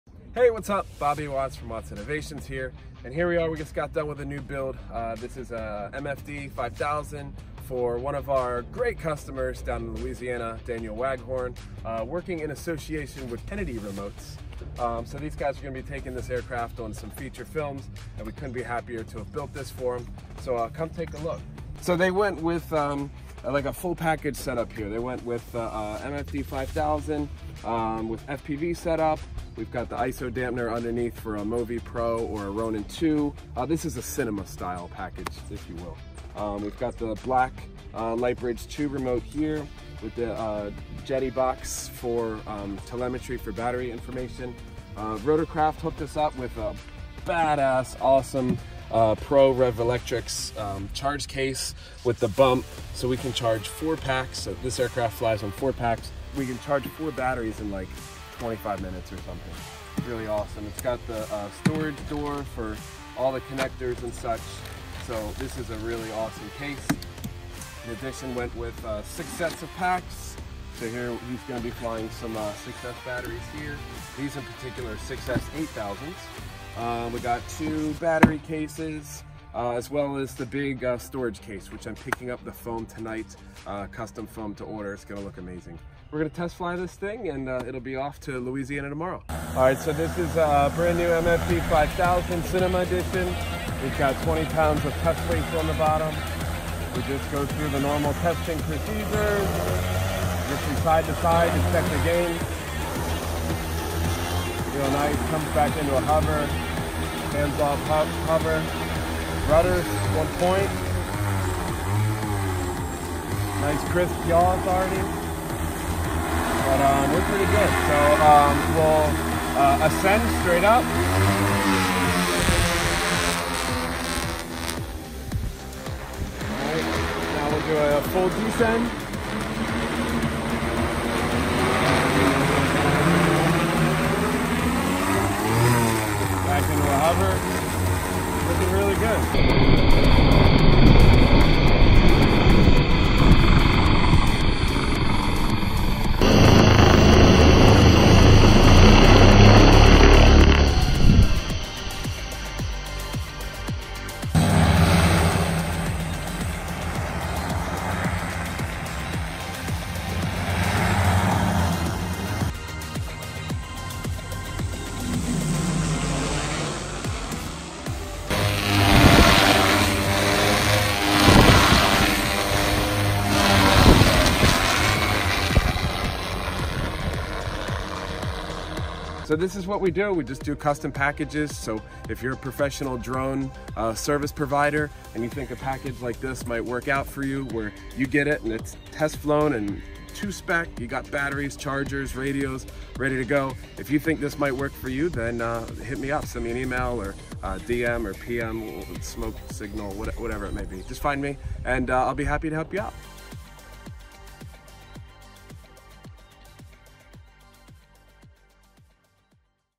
Test Flying a New MFD 5000 Cinema Edition Drone Build For a Customer
Just completed a new build for a great customer Daniel Waghorne / Kennedy Remotes down in Louisiana. We did a custom Juggernaut themed MFD 5000 Cinema Edition utilizing the A3 Flight Controller with Lightbridge 2. For batteries and charging, we went with sets of 4x 6s 8,000mah Tattu LiPo's and an awesome custom Rotorcraft Pro Revolectrix Bump case.

Like always, just testing with a dummy payload- in this case 20lbs.

This aircraft will fly the DJI Ronin 2 or Freefly Movi Pro gimbal. Just getting the initial test flights on it before shipping out... couldn't have gone any smoother!